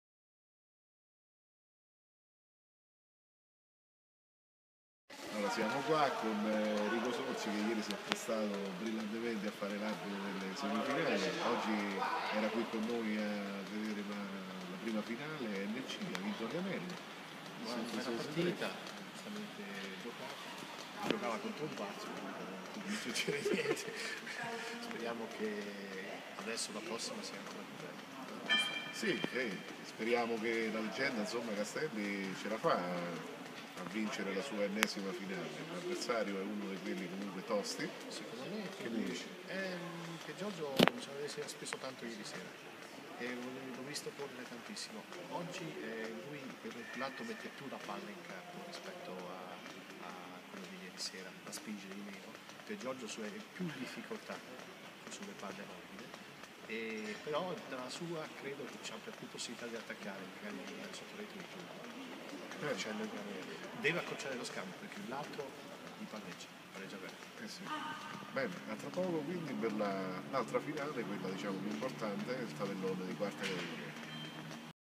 Intervista ad Enrico Sorci
Pareri sulla finale NC appena conclusa e presentazione tecnico/tattica di quella di 4° cat.